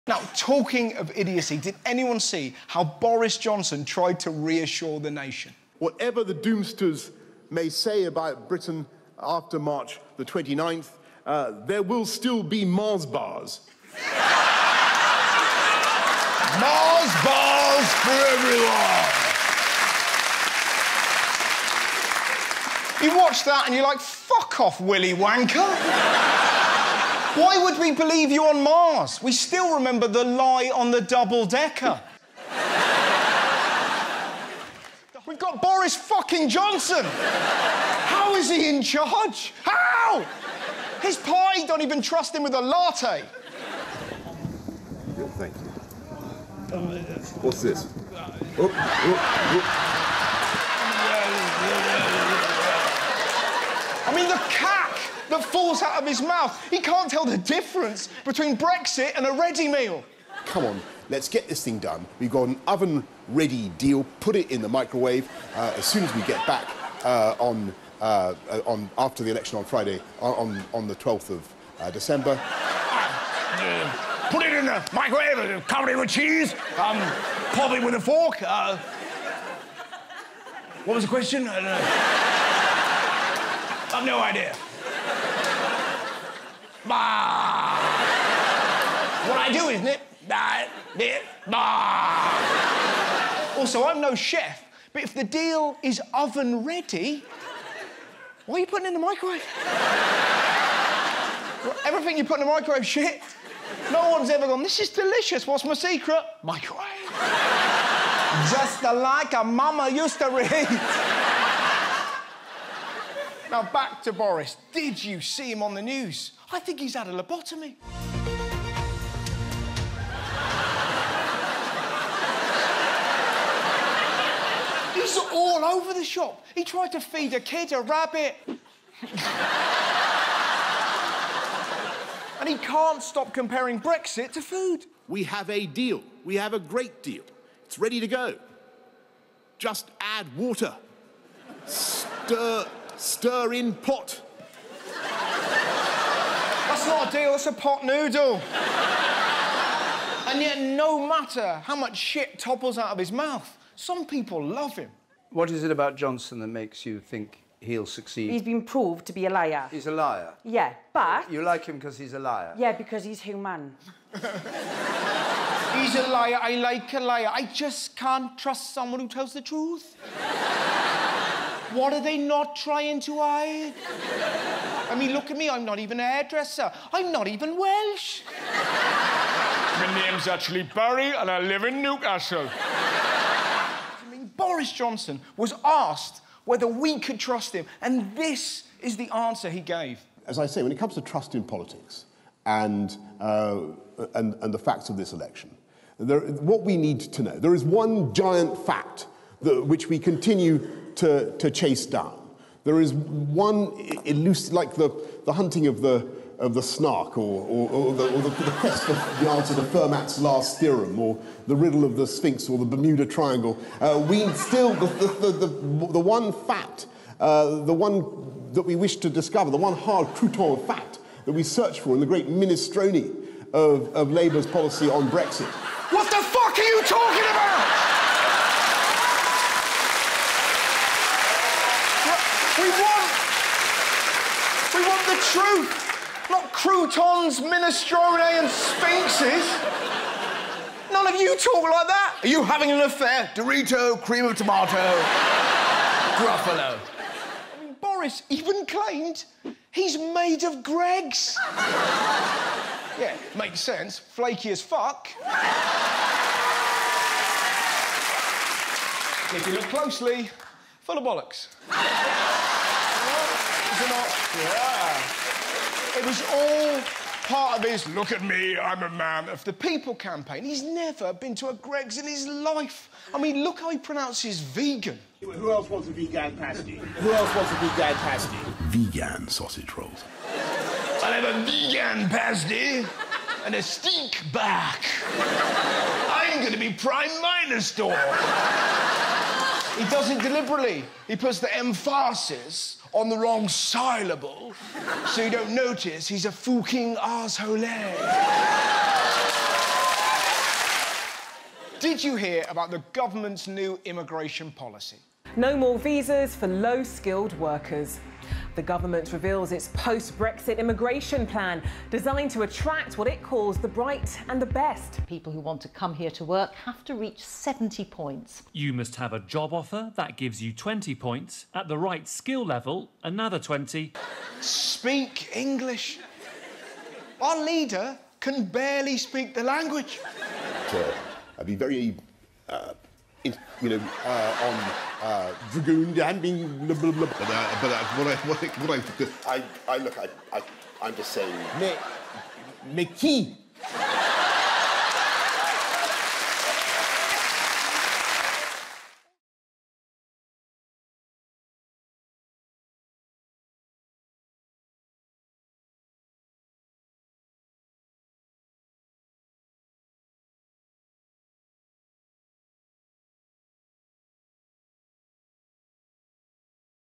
Why We Shouldn't be Surprised Boris is So Sh*t | The Russell Howard Channel
Boris Johnson has proved to be truly awful, but we can't say we didn't see this coming...